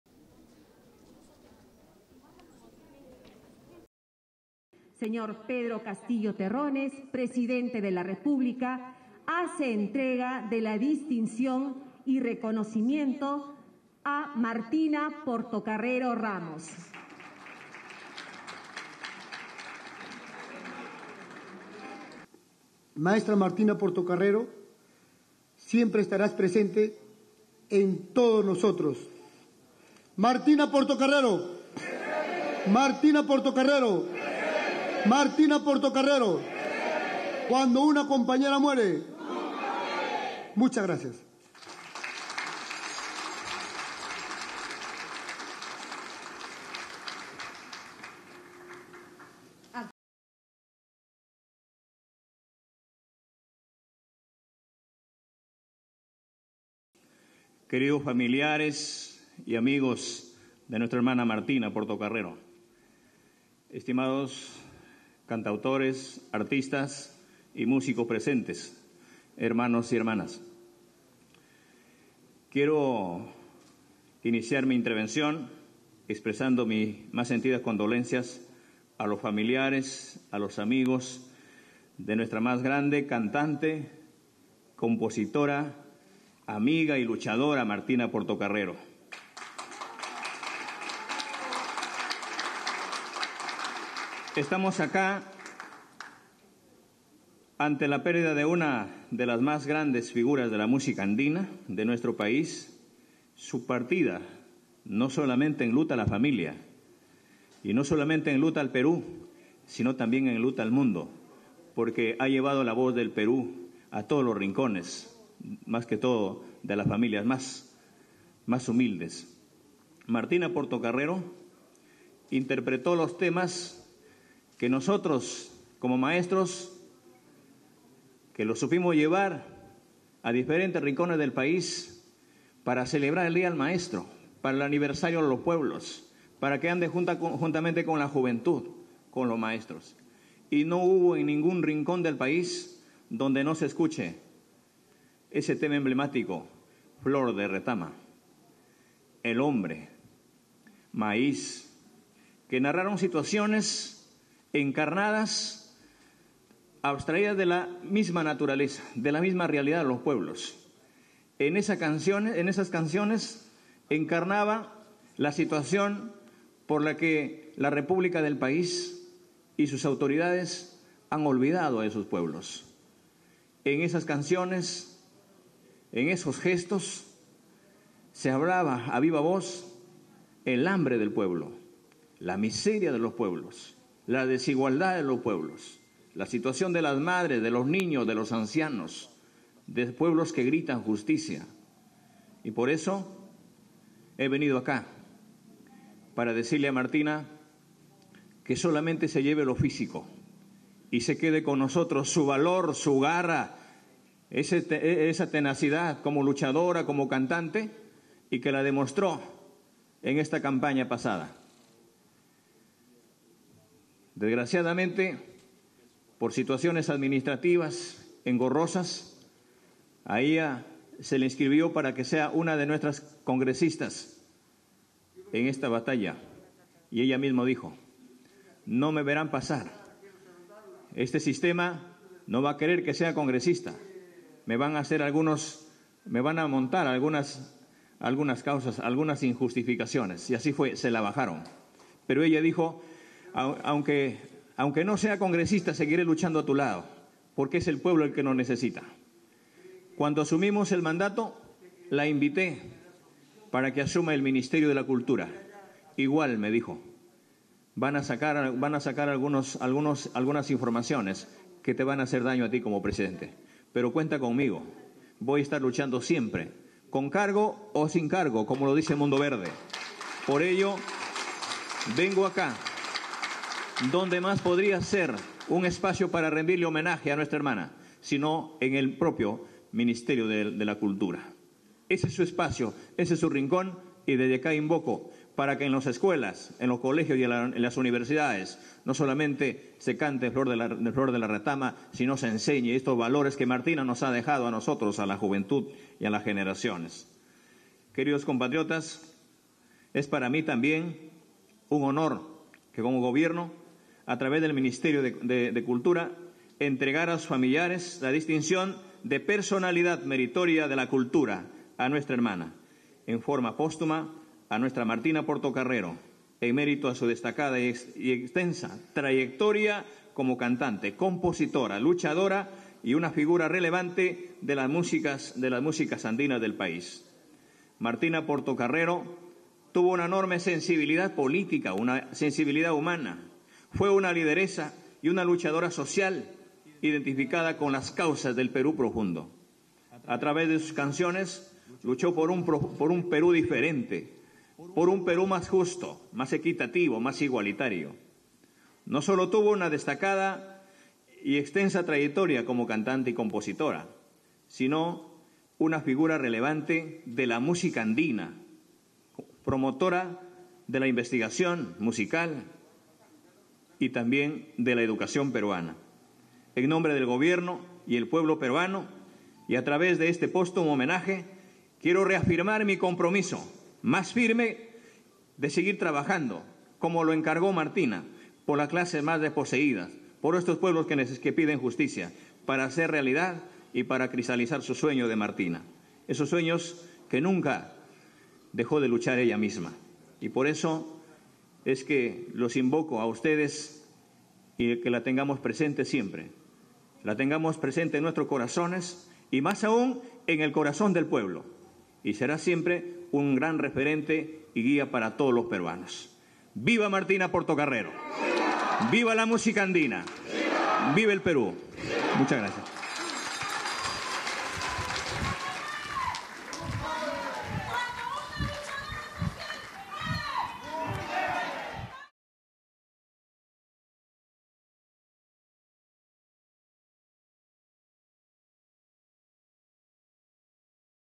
Homenaje póstumo a la cantante Martina Portocarrero
El presidente de la República, Pedro Castillo, participó en el homenaje póstumo a la cantante y compositora Martina Portocarrero en la sede del Ministerio de Cultura.